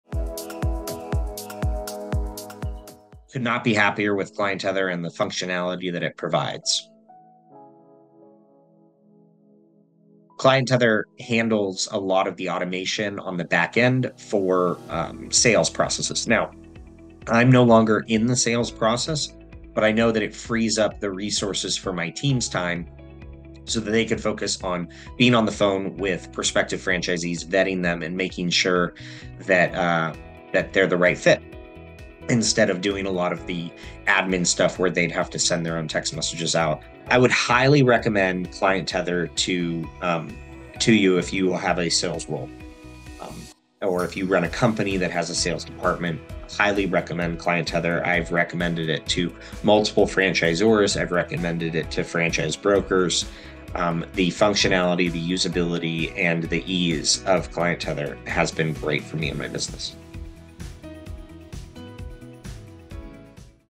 Overwhelmed by Your Sales Process? Here's an Easier Way!
📱 Sales taking up too much of your time? It doesn’t have to be that way! Aaron Harper, owner of Rolling Suds, found the perfect solution to automate his sales process and save countless hours of work – ClientTether.

In this testimonial, Aaron walks us through the many ways ClientTether’s texting and automation capabilities have transformed his business. From automatically following up with potential franchisees to managing communication across the company, Aaron’s team can now focus on what really matters: building relationships and growing the business.

Rolling Suds, the largest power washing franchise with 197 locations across 27 states, is a huge success, and Aaron credits a lot of that success to the efficiency that ClientTether brings. No longer bogged down by administrative tasks, his team is free to do what they do best: connect with new prospects and ensure franchisees are supported every step of the way. Aaron explains how ClientTether doesn’t just handle sales – it also allows his operations team to track every call, meeting, and coaching session, ensuring everyone stays on the same page.

If you’re looking for a way to streamline your sales process, reduce admin time, and focus on growing your business, ClientTether could be the perfect solution. Aaron highly recommends it to anyone with a sales team or a business that needs to be more efficient. Watch now to see how ClientTether has made a difference for Rolling Suds and how it could work for you too! 🌟📊